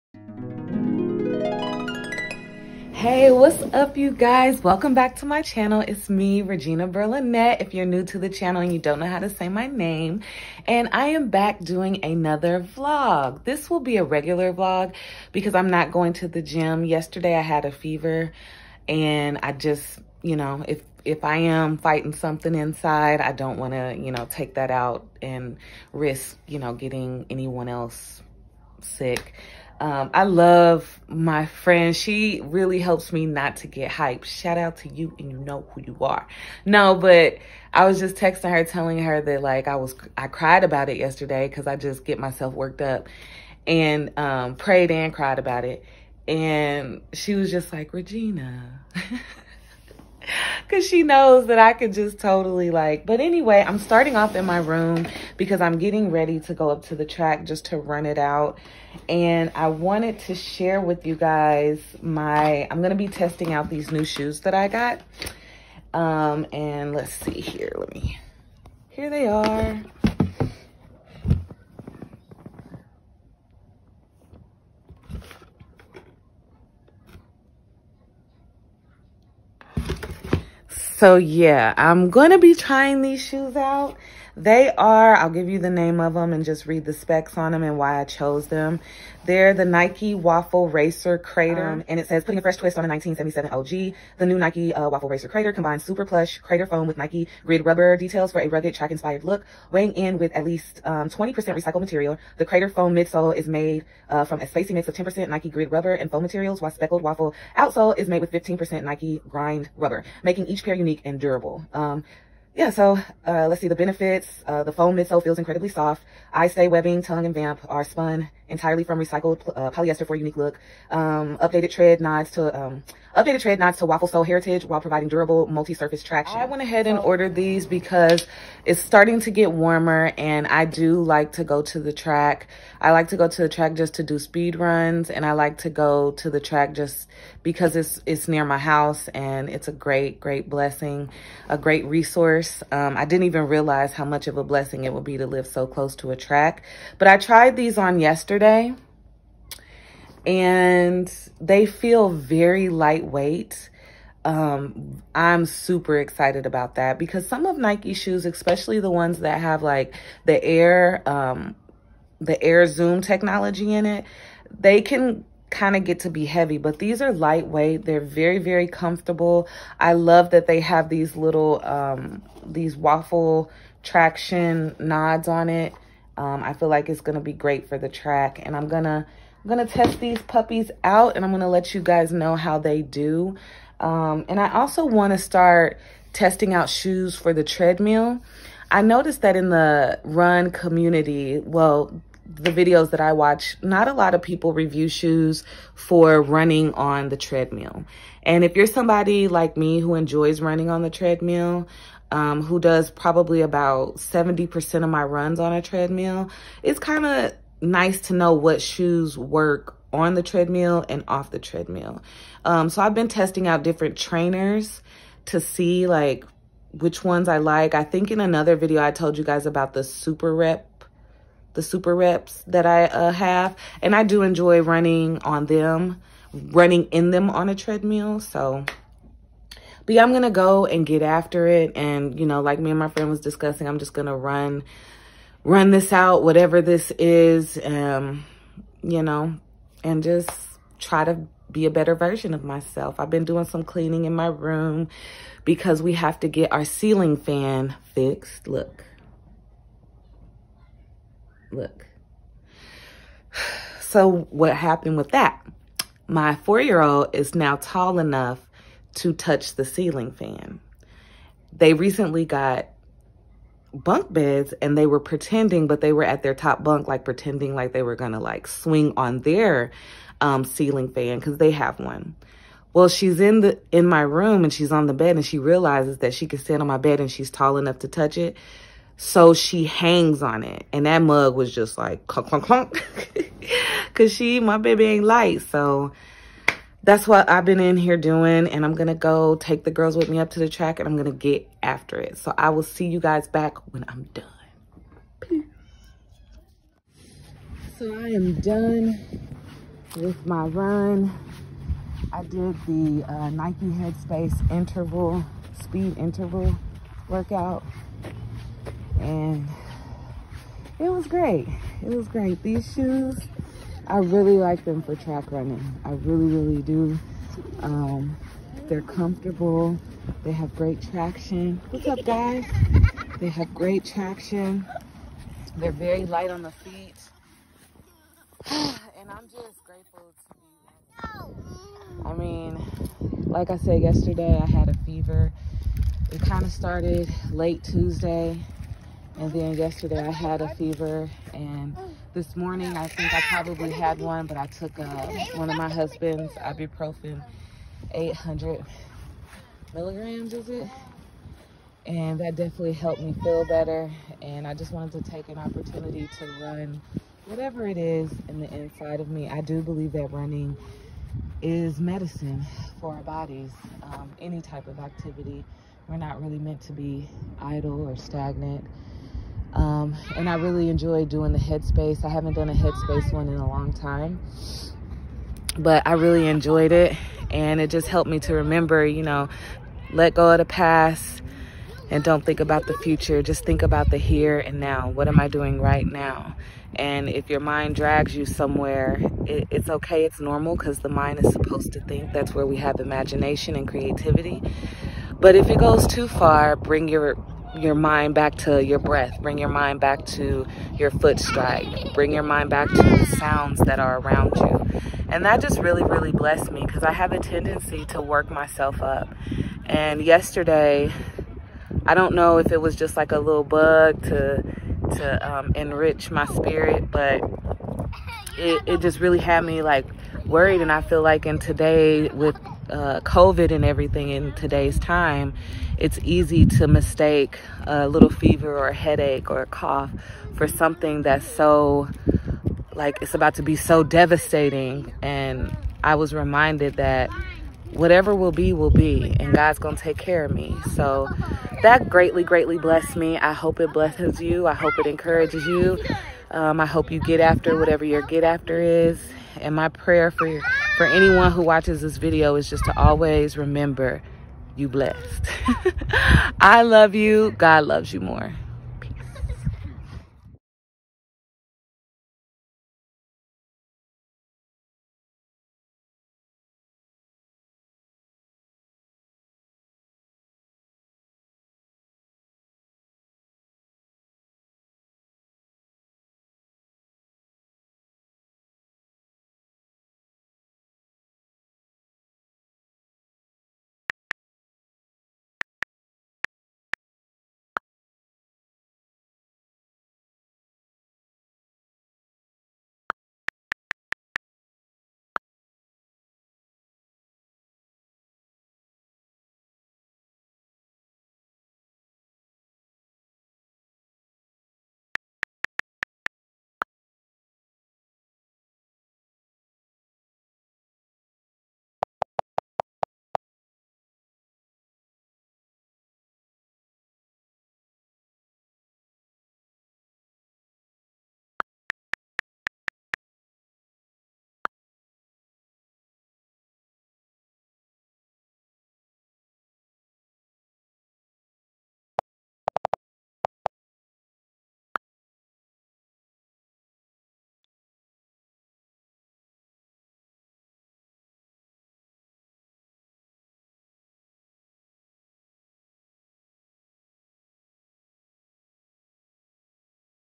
Friday | March 19, 2021
Hey! I hope you’re well and in good health. Today’s vlog is a little different... Meaning it’s not a car chat! I actually haven’t been feeling all that well the last 2 days! And it was my first time having a fever sense the pandemic so I was tripping a little bit... Ok a lot. Had a good cry about it. Most importantly prayed about it and now I’m better! A good reminder for me to remember to not catastrophize... Hey, I’m an extremist! How do you handle illness especially during this time? Leave it in the comments if you like.

“He said not ‘Thou shalt not be tempested, thou shall not be travailed, thou shalt not be dis-eased’; but he said, ‘Thou shalt not be overcome’.” Julian of Norwich,  Revelations of Divine love